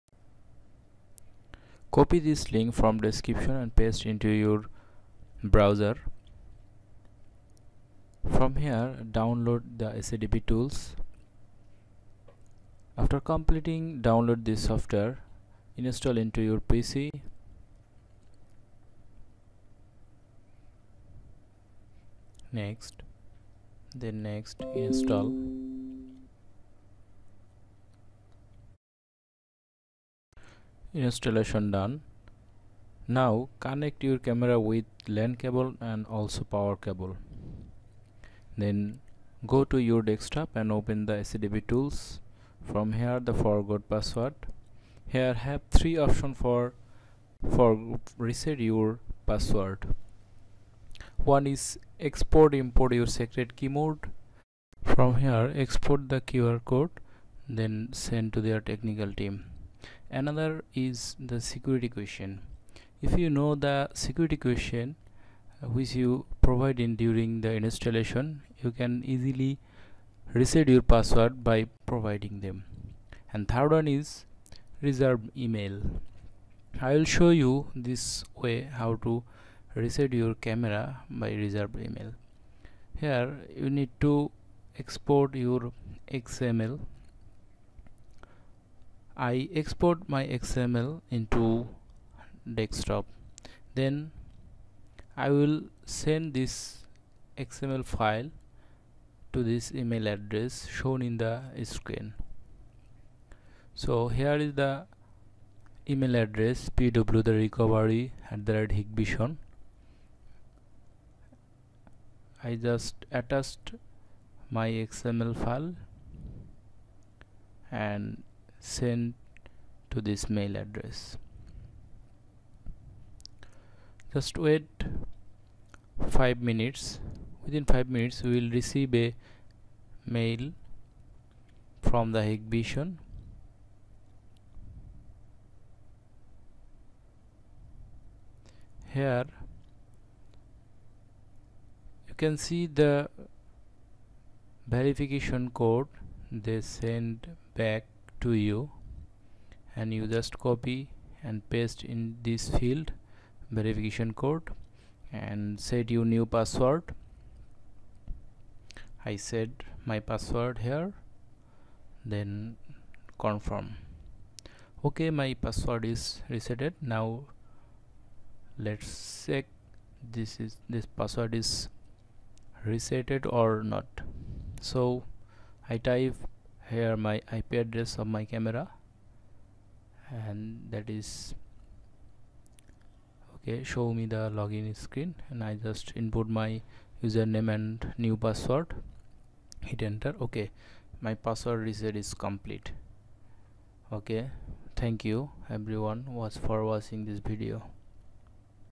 How to reset hikvision IP camera password
How to reset hikvision IP camera password using SADP

Tuts Share is a Tech Tutorials & Travel Vlog Sharing Channel.